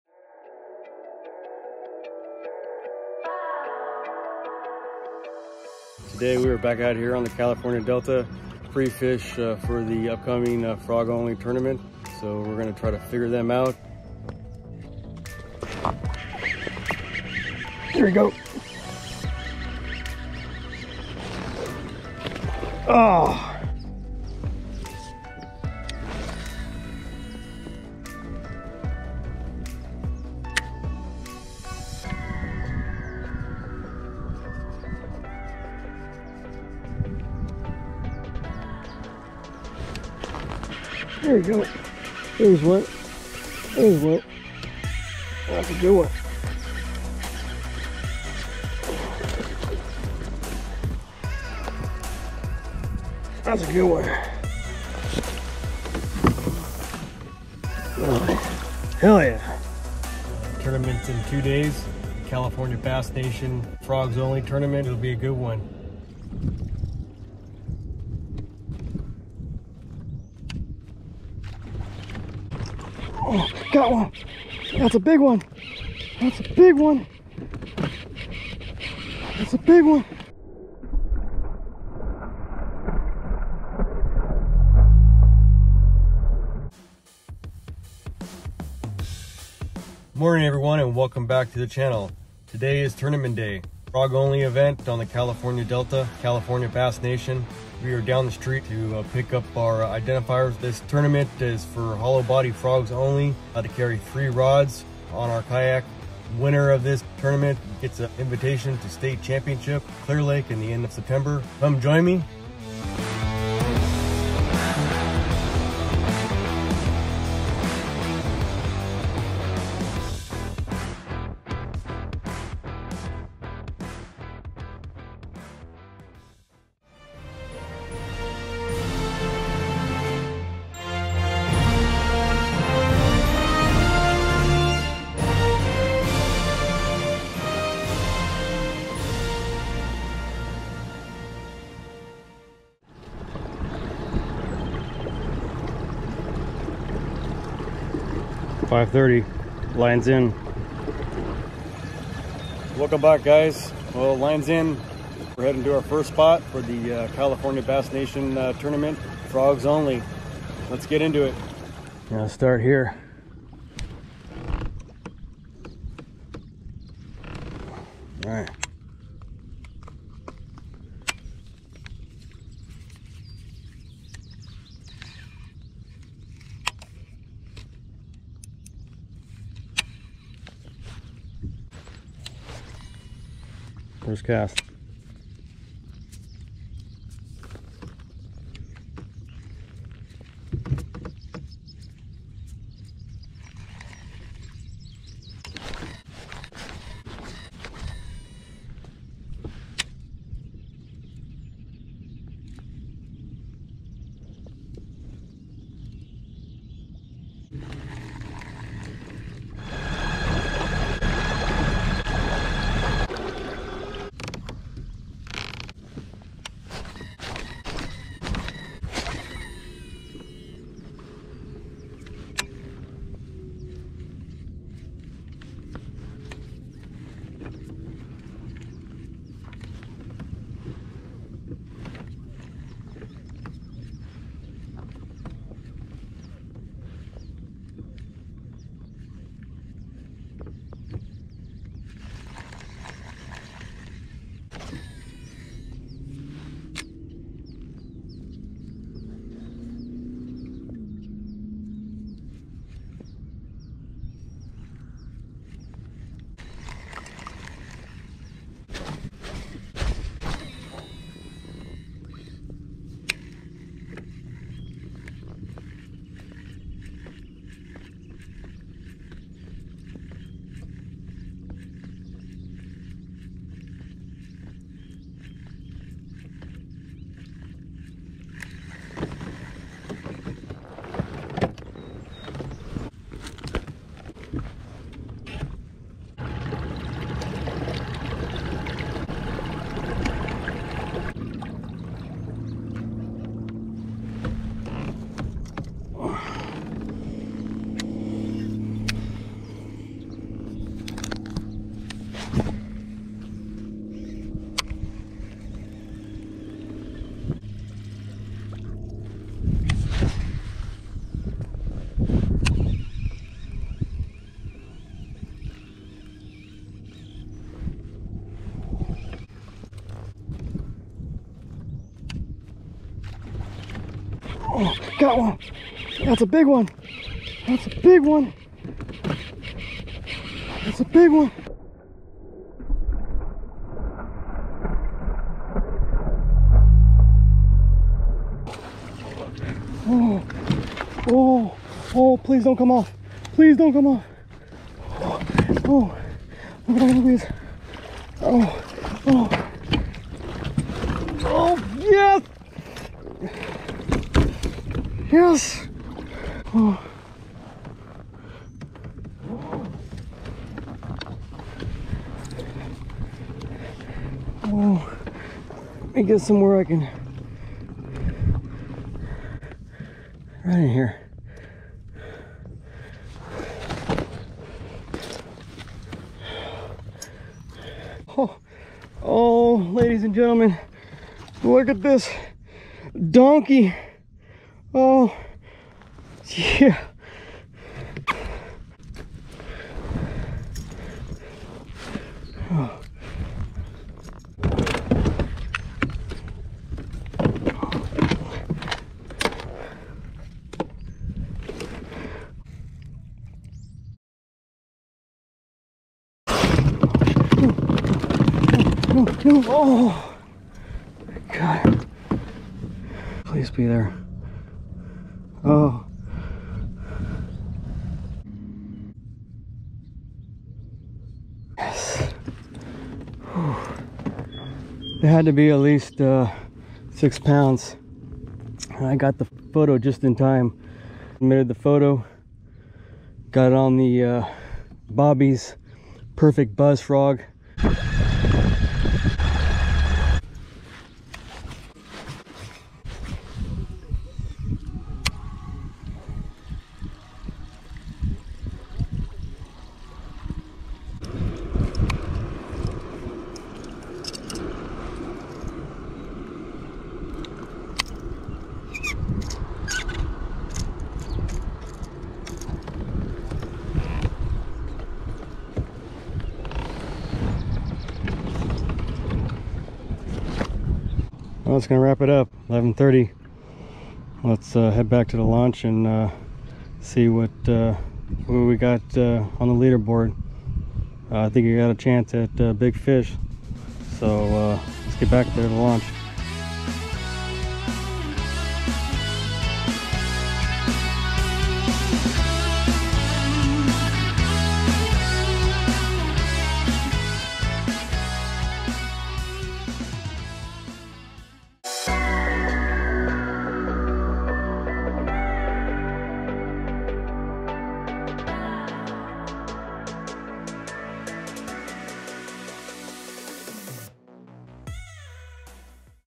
California Delta Bass Fishing | FROGS ONLY Tournament
2022 Frogs Only Bass Fishing Tournament 2nd Annual Ca Bass Nation. Remastered Video, Big Bass Winner!